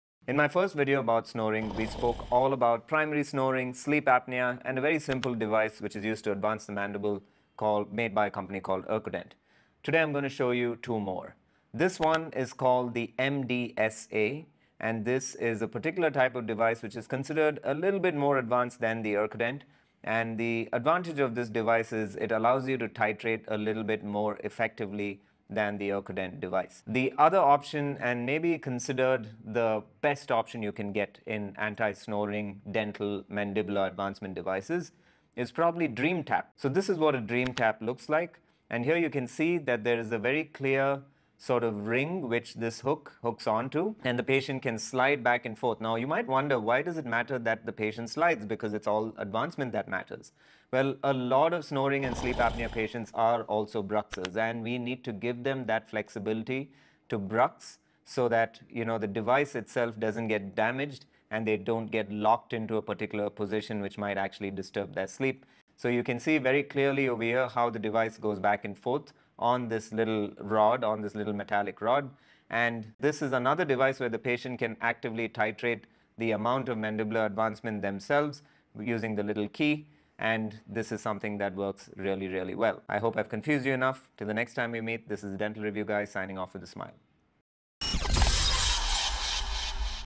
Dental Anti-Snoring devices Part 2
Let's look at some more advanced anti snoring devices - the MDSA and the DreamTap! If you're a dentist and not providing these simple life changing devices to your patients with snoring and sleep apnea, it's time to get yourself educated and start right away!